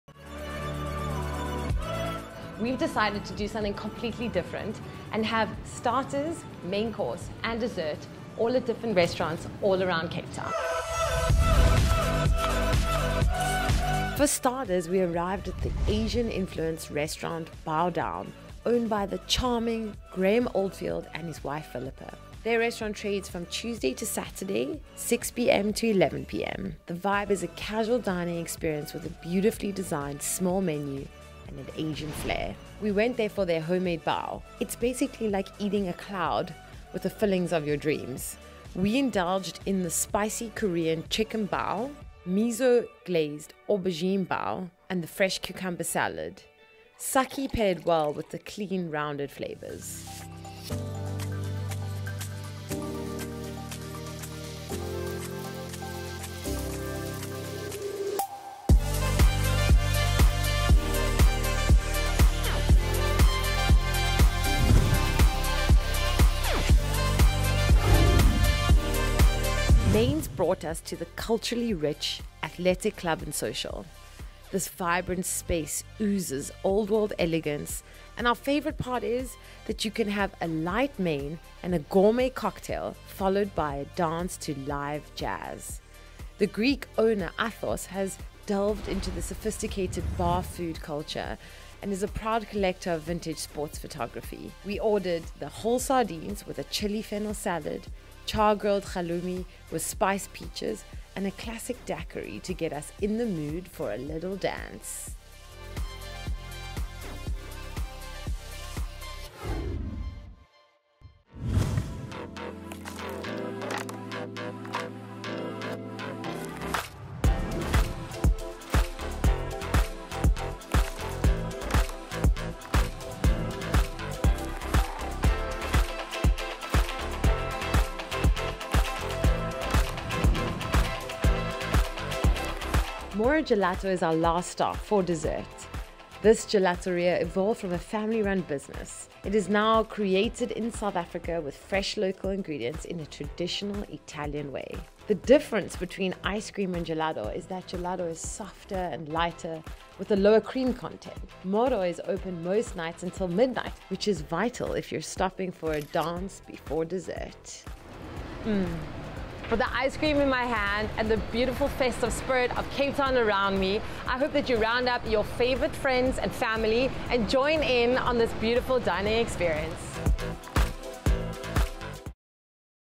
From starters to desserts in Cape Town
WATCH: Three Cape Town locations. Let's check out their starters, mains and desserts. From Asian fusion and old-world eligance to classic Italian.